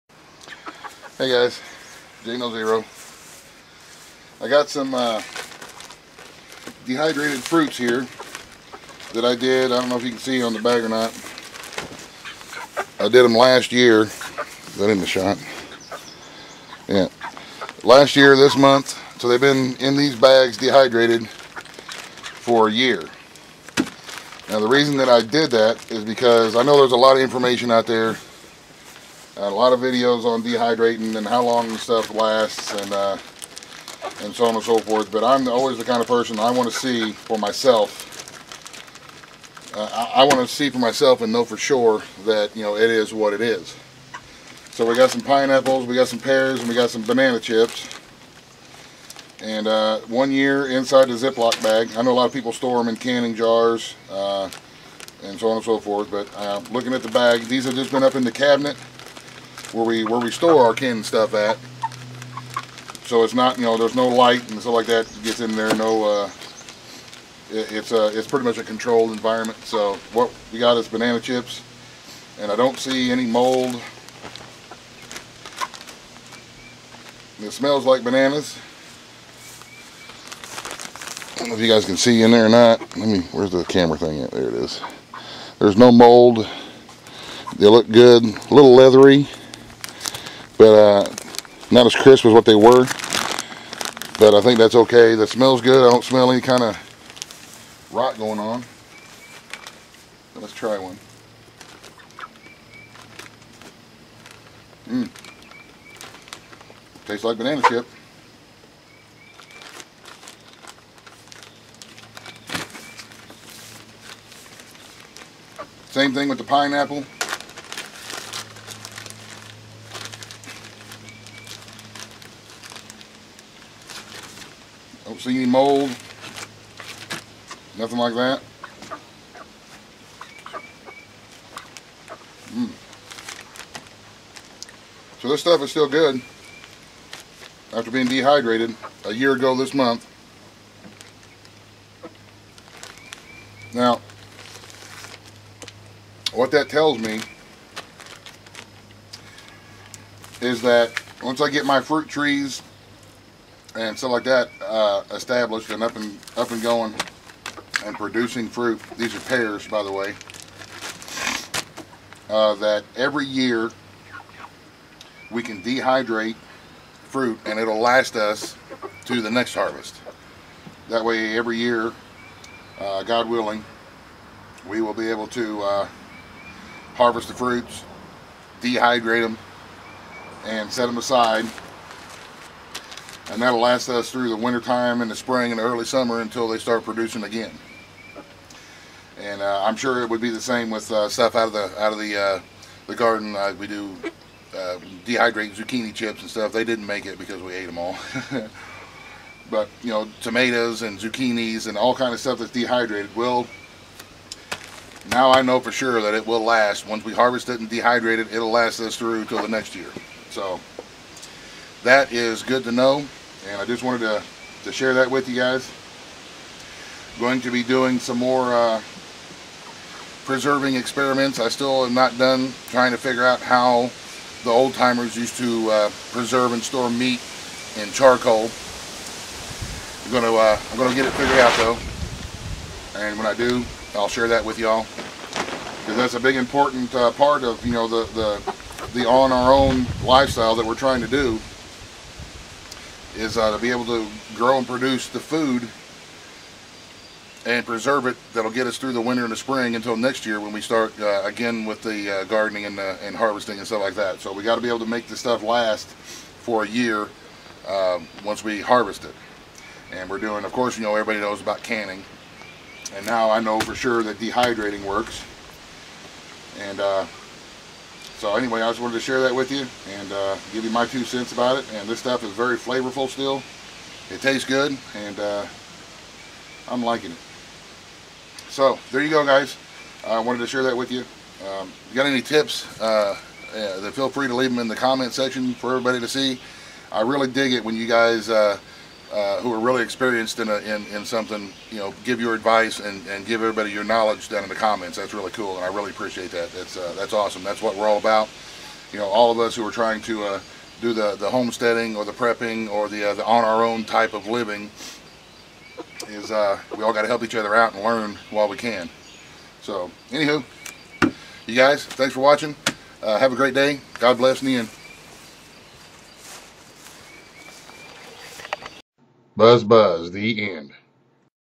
Dehydrated fruits one year later
Hey guys, I dehydrated some fruits one year ago and now I know for certain that our harvest will be preserved until the next annual harvest.
J